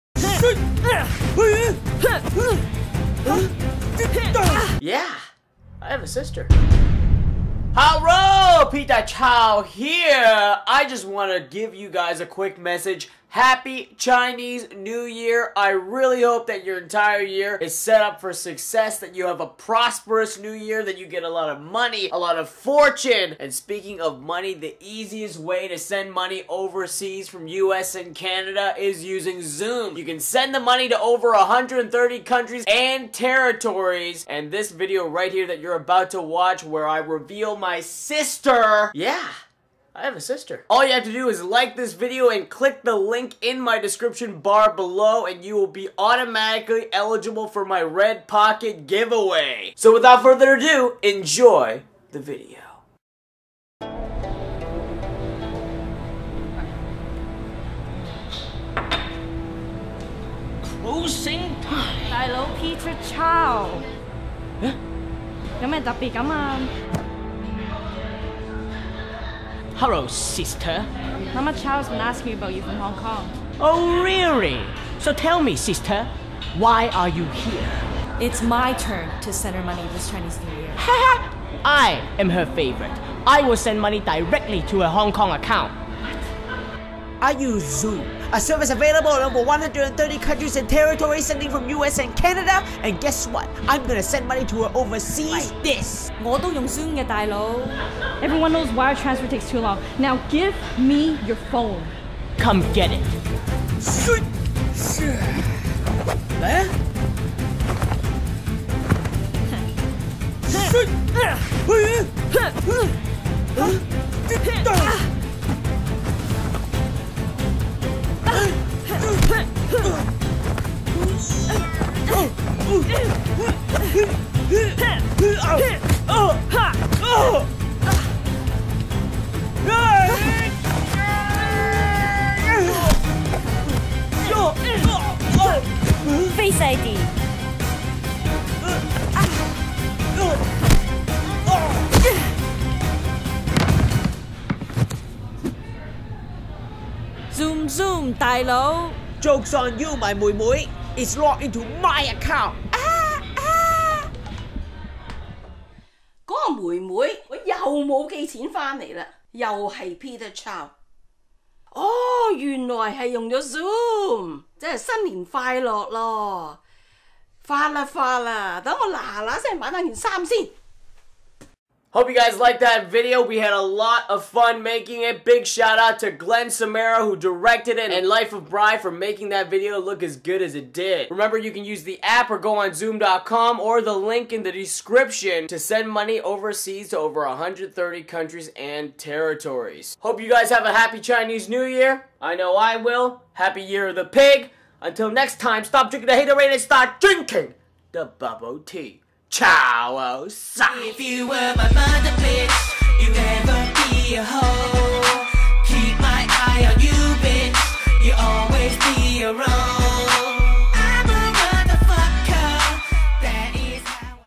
Meeting My Sister For The First Time!
Peter Chao confronts his sister on who gets to use Xoom to give Mama Chao a red pocket for Chinese New Year.

▶ Mama Chao as Mama Chao